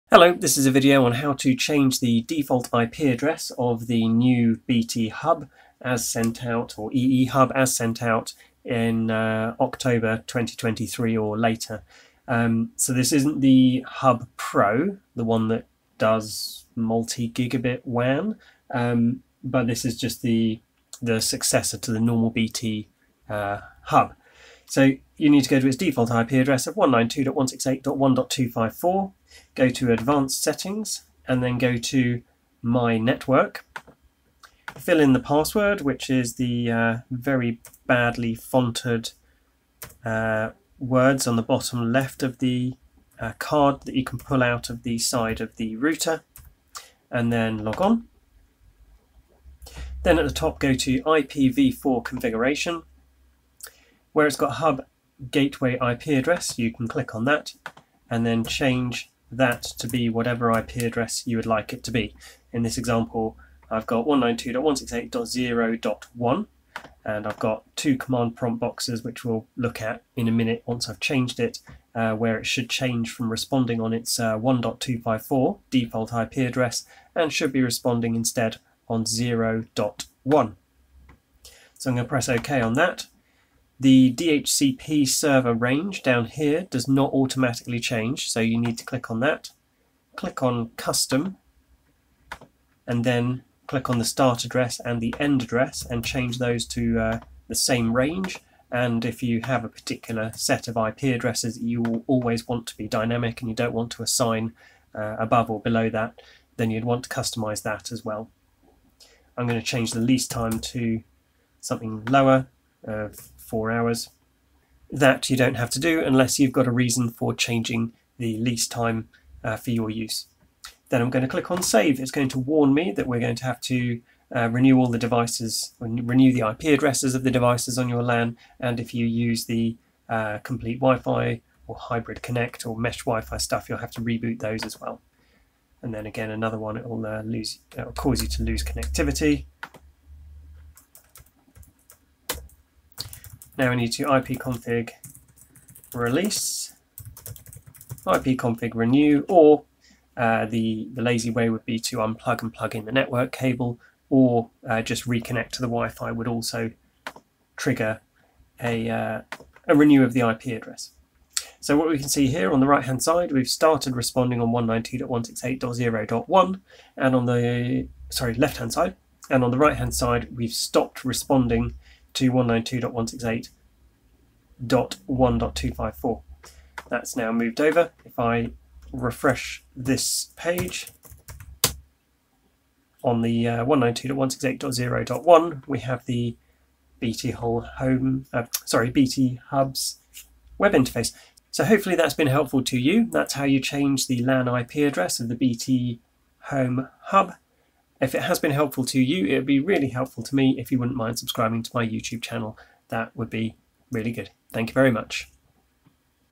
How to change the BT Home Hub 2023 LAN IP address.
Quick instructions on how to change the LAN Gateway IP of the BT Home Hub Router and DHCP range.